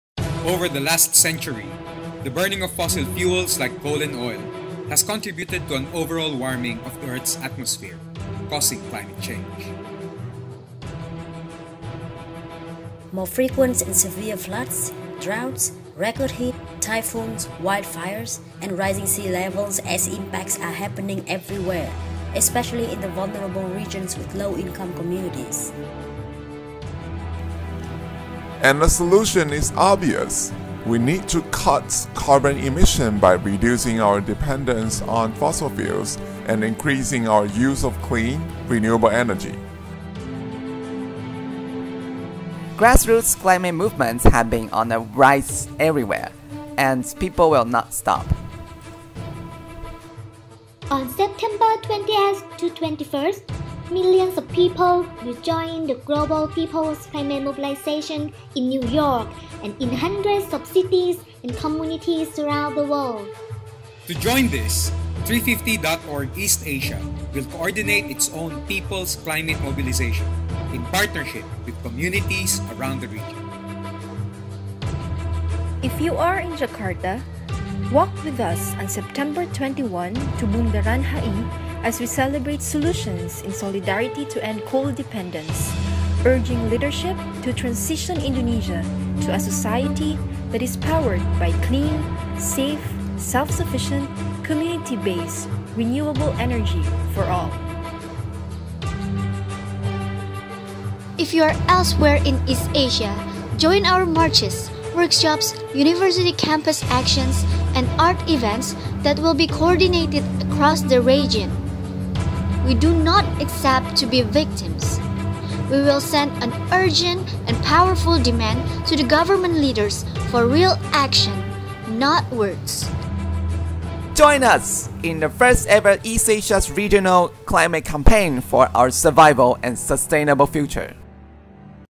Peoples Climate March | East Asia
Our demand is for Action, Not Words: take the action necessary to create a world with an economy that works for people and the planet – now. In short, we want a world safe from the ravages of climate change.